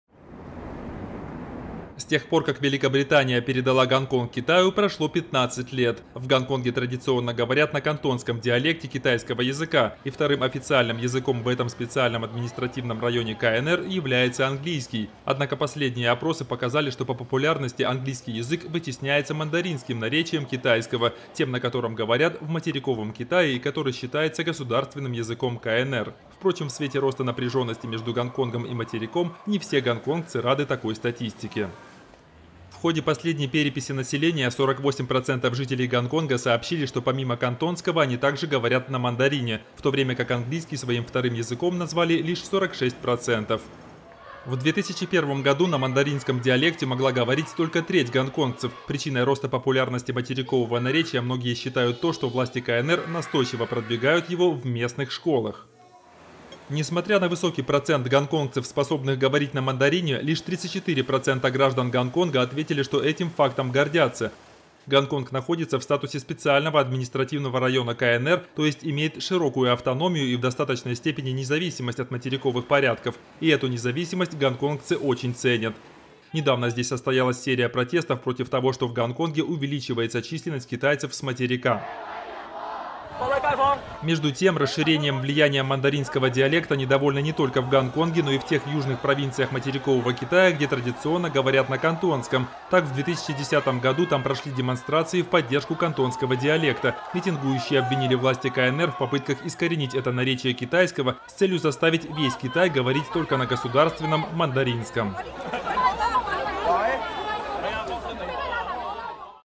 В Гонконге английский вытесняется мандаринским
) В Гонконге английский язык, который здесь считается вторым официальным, вытесняется мандаринским китайским, который является официальным языком КНР. Об этом свидетельствуют последние опросы населения. При этом многие гонконгцы не рады такой статистике, считая, что таким образом власти КНР пытаются увеличить свое влияние в Гонконге.